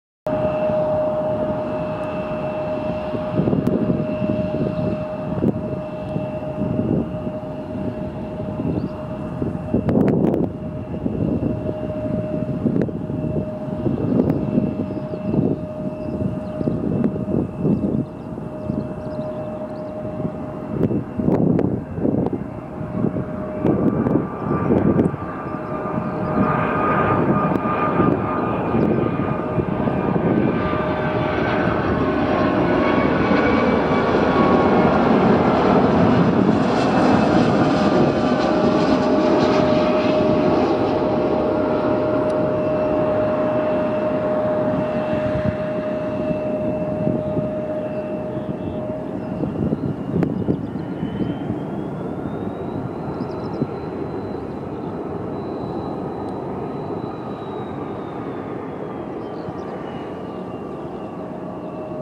Icelandair Flight 494 (Raykjavik to BHX)
Icelandair Flight 494 from Raykjavik to Birmingham Airport. The aircraft is an Boeing 757-225(WL) (reg: TF-FIA and named Herdubreid) is 15.6 years old and has been operating for Icelandair since 2007.